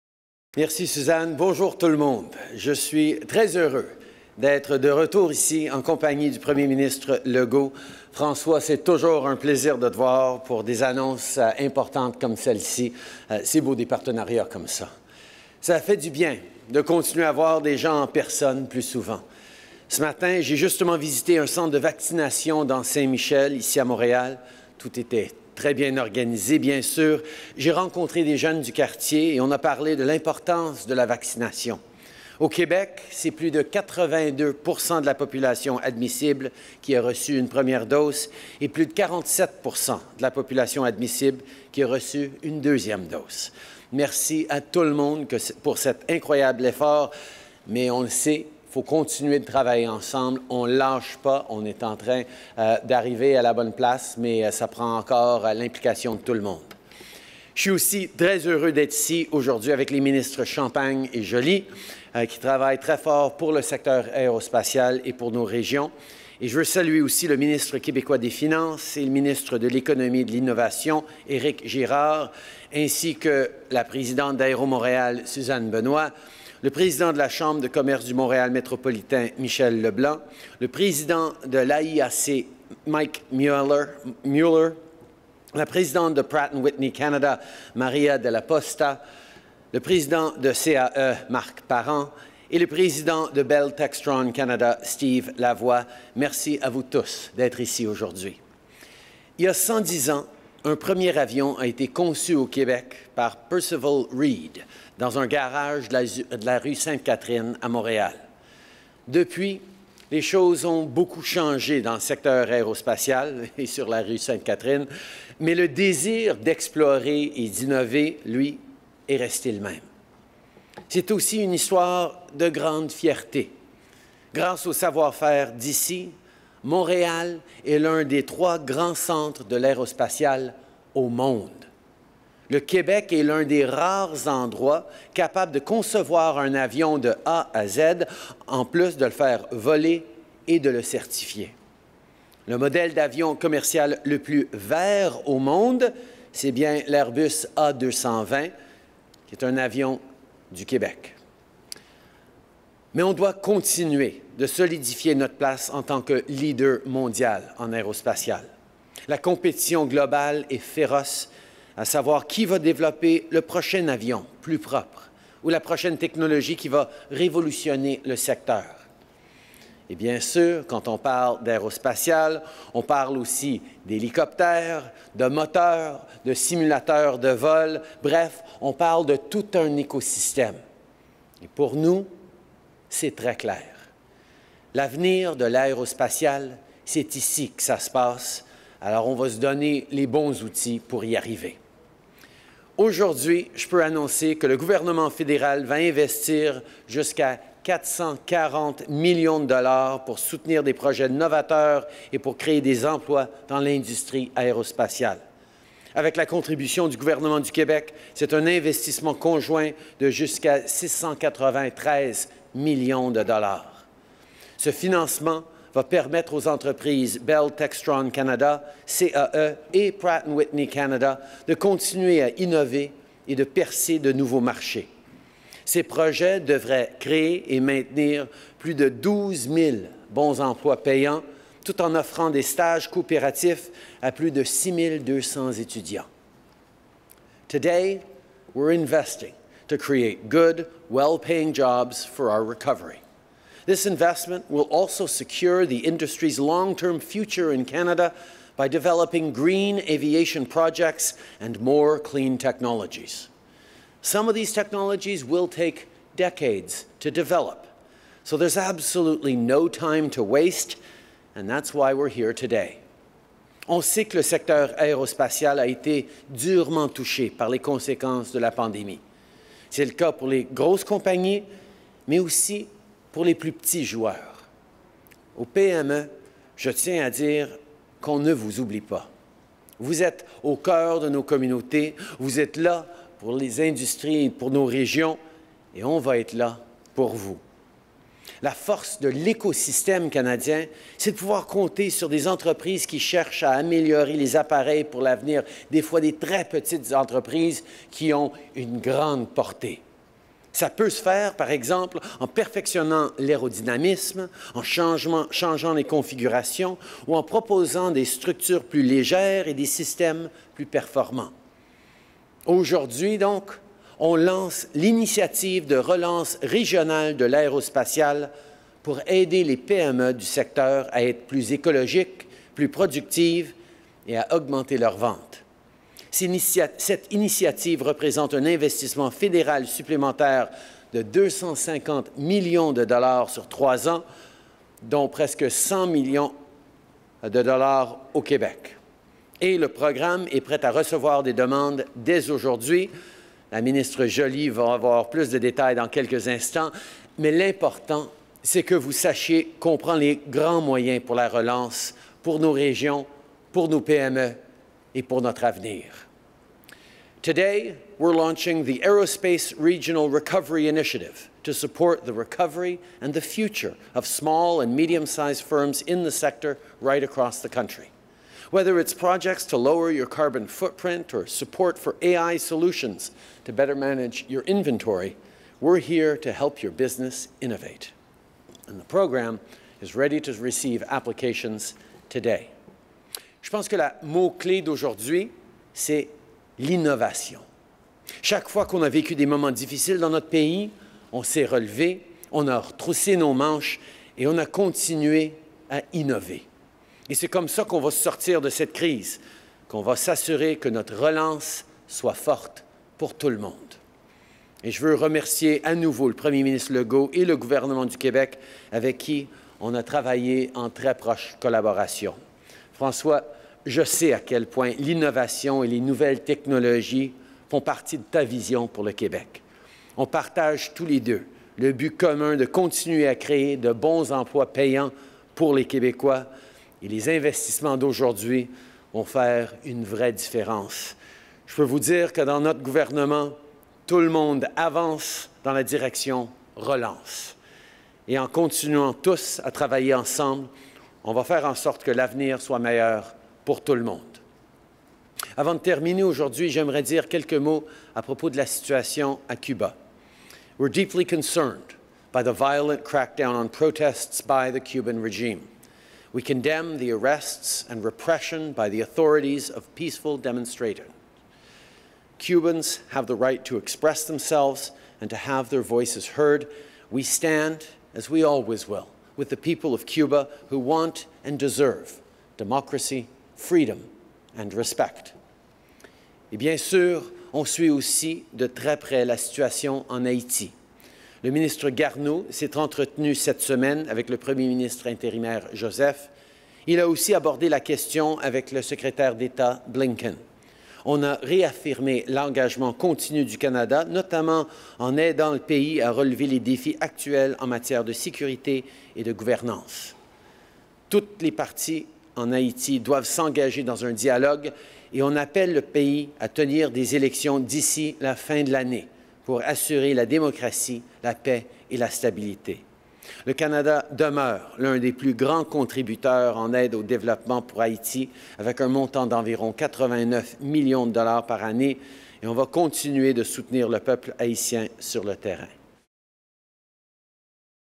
Remarks announcing new investments to strengthen Canada’s aerospace sector
Prime Minister Justin Trudeau delivers remarks announcing new investments to strengthen Canada’s aerospace sector.